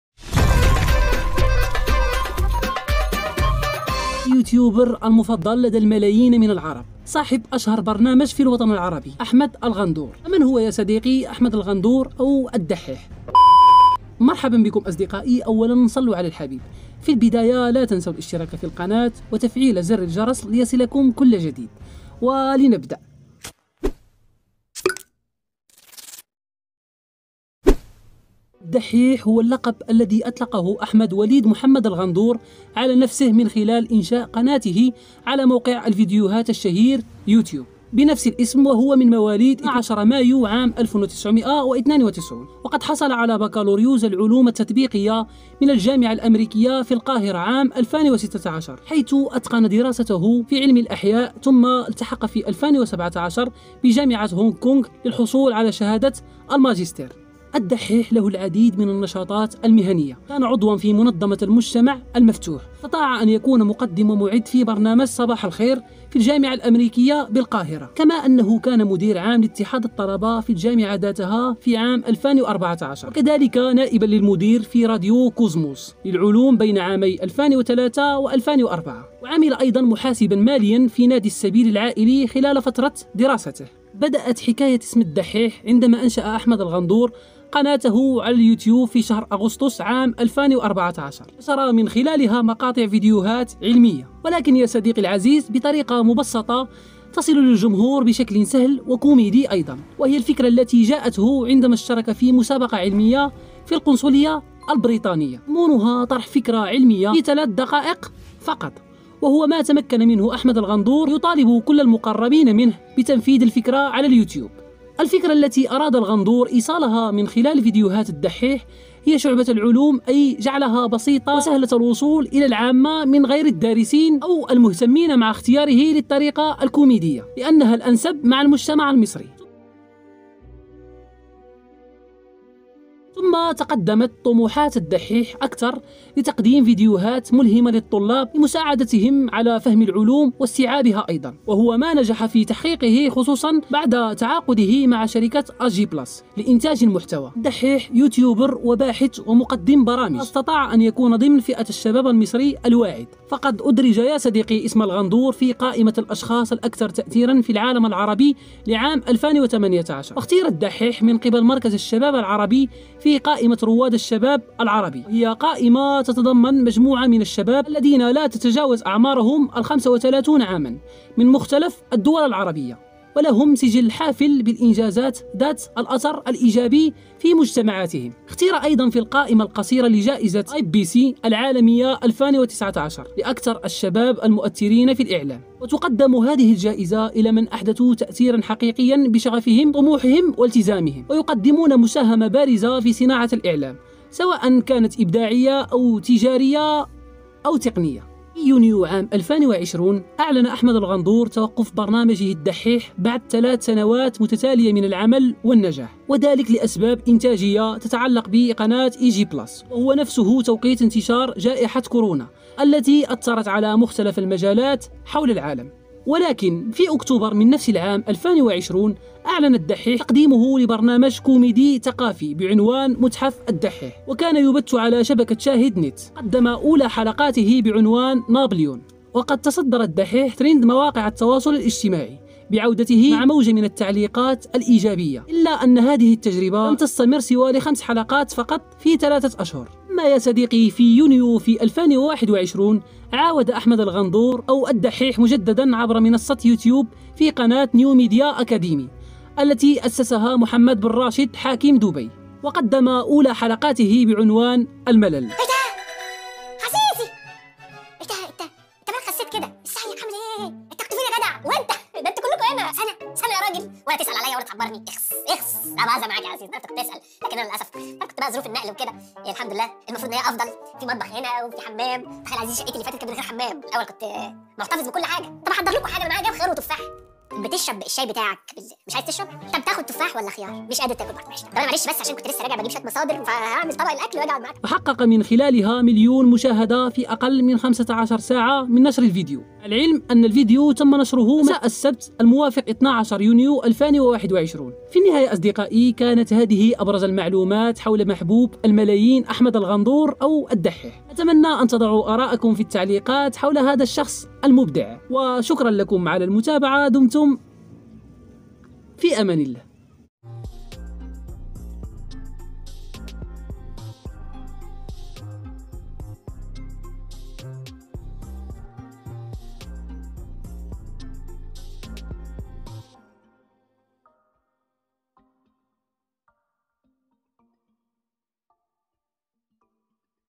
من هو الدحيح أو احمد الغندور | اليوتيوبر الاشهر في الوطن العربي 😎 وهل هو فعلا ملحد!!! محمد جميعي
ولمن لا يعرف من هو «الدحيح»، هو شاب مصرى نابه اسمه أحمد الغندور، هو باحث وصانع محتوى فيديو، اشتهر باسم «الدحيح» نسبةً لاسم قناته على اليوتيوب، وهو أحد البارزين فى مجال تبسيط العلوم. تمكن من أن يكون أحد أهم صناع المحتوى العرب.
محمد جميعي

محمد جميعي
Mohamed jami
اتمنى ان نصل الى 10000 مشترك بفضل الله ثم بفضلكم نستطيع فعلها😍.